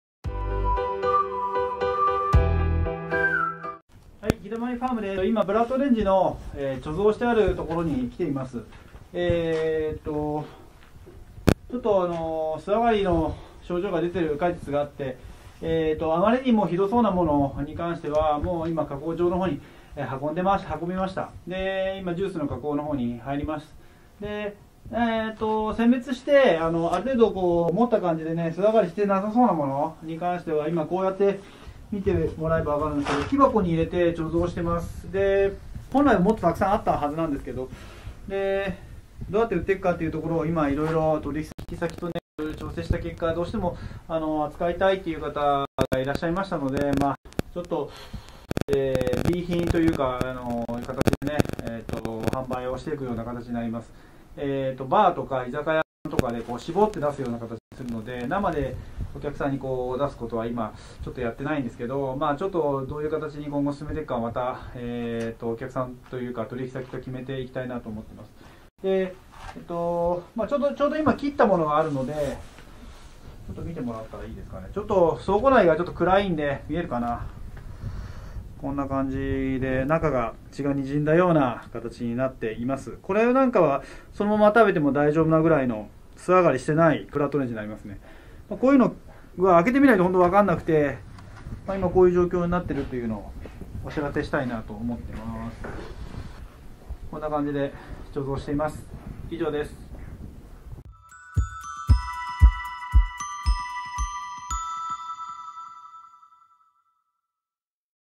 ブラッドオレンジを貯蔵しています。【大寒波でほぼ全滅】大切に育ててきたブラッドオレンジがほぼ全滅してしまいました…
2021年、大切に育ててきたブラッドオレンジが大寒波によって落下、す上がりの被害に遭い、ほぼ全滅してしまいました。
私たち農家は、太陽の光、水、大地と空気を利用して作物を育てていますので天候の影響はそのまま作物に伝わります。
今年の収穫量が少なくなってしまいました。
すでにご注文を多数いただいておりますが、すべてのお客様に
お届けできないかもしれません。予めご了承ください。